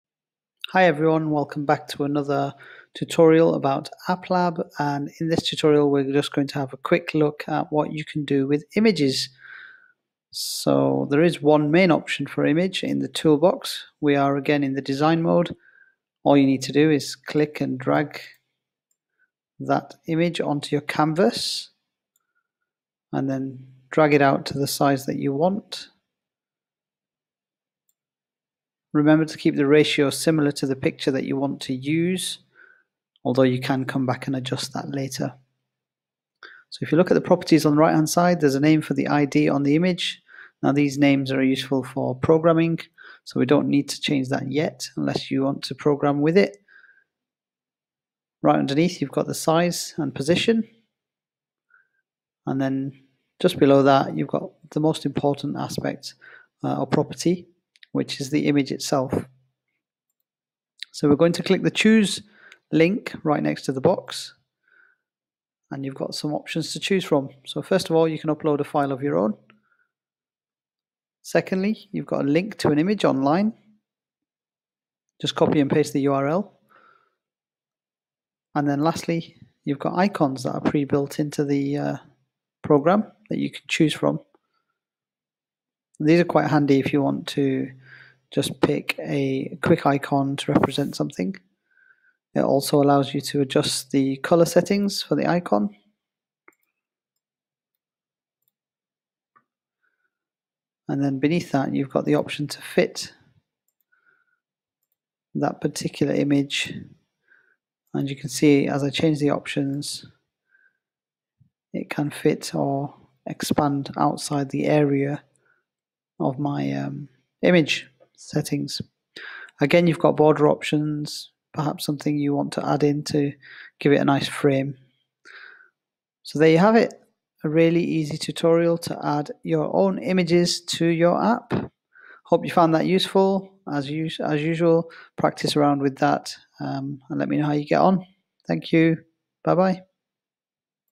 03. App Lab - Images
This tutorial will show you how to add an image and change the main properties.